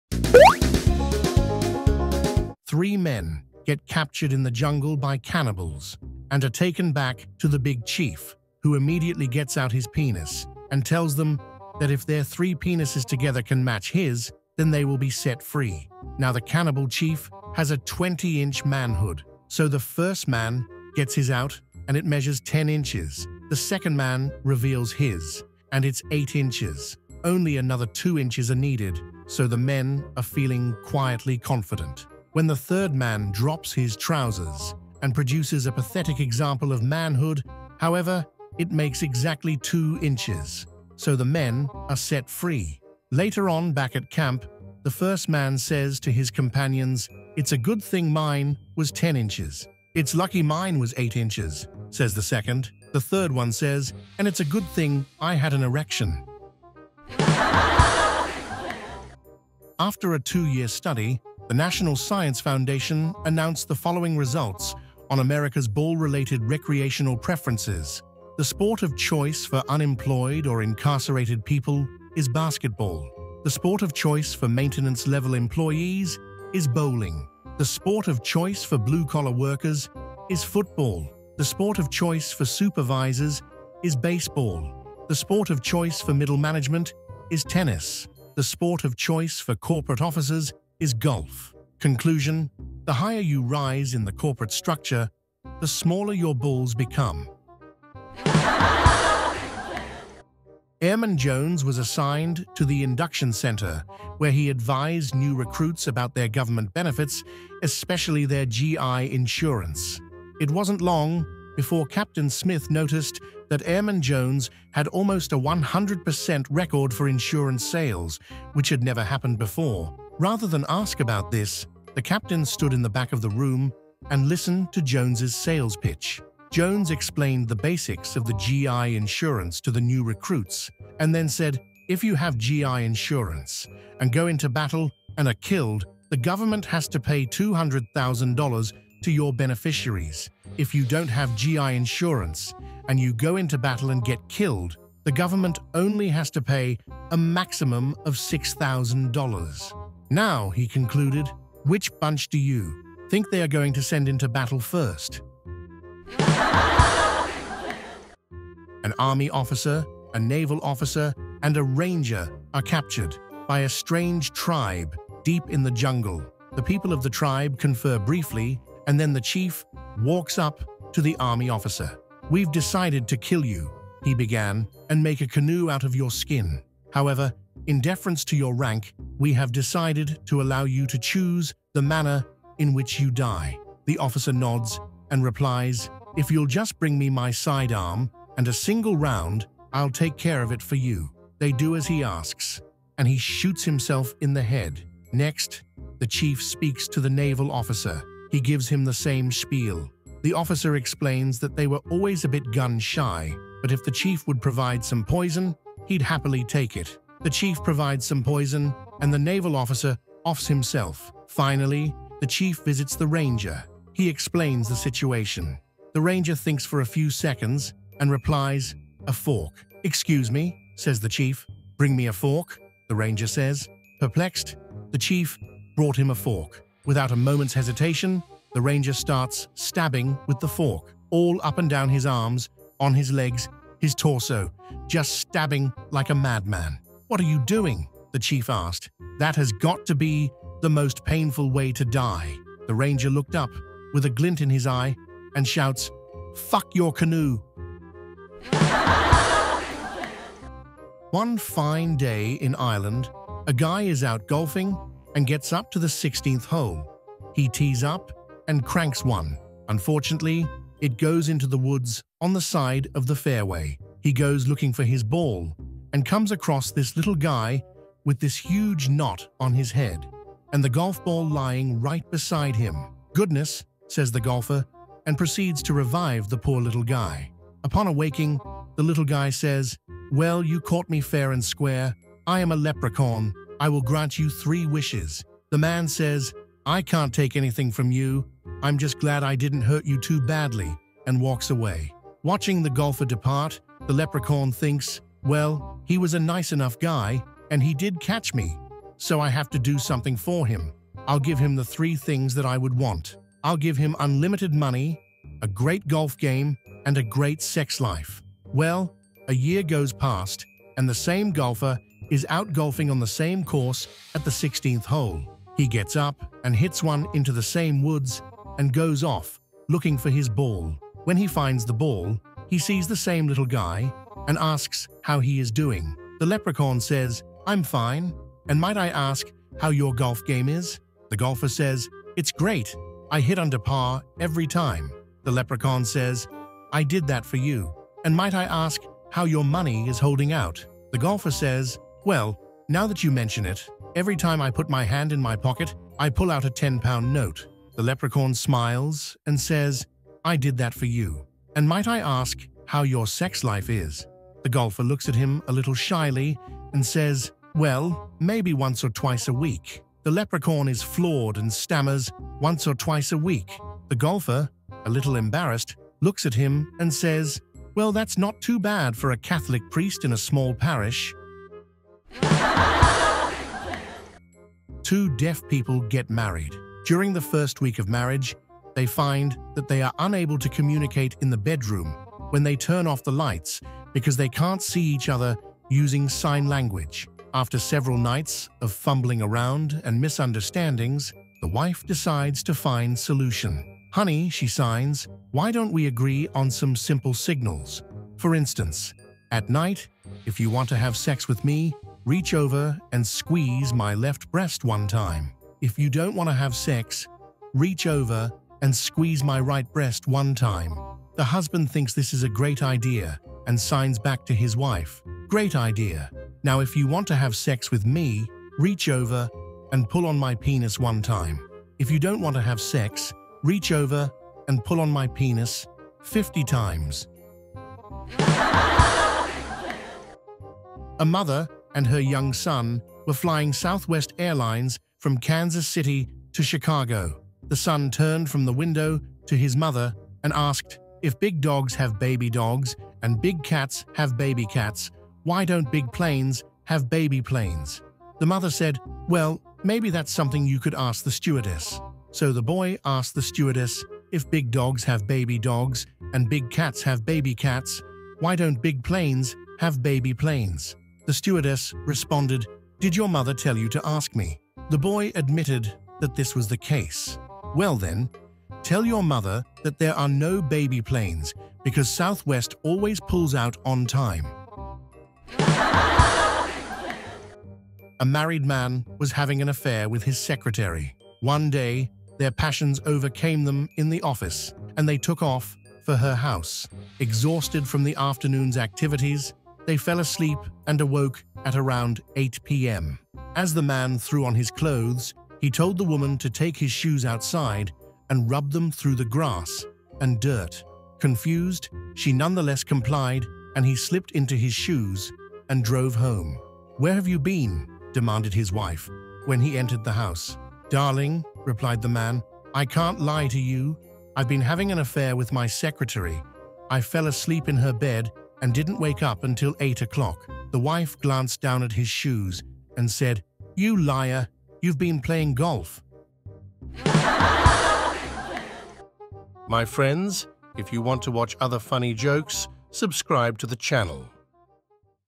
Best Joke Of The Day. 105. Three men get captured in the jungle by cannibals ...
LAUGHTER EXTENDS LIFE.
BEST JOKES EVERY DAY.

Timestamps:
00:00 Joke 1
00:59 Joke 2
01:40 Joke 3
02:38 Joke 4
04:10 Joke 5
06:14 Joke 6
07:10 Joke 7
07:57 Joke 8
08:47 Advertising
08:55 The end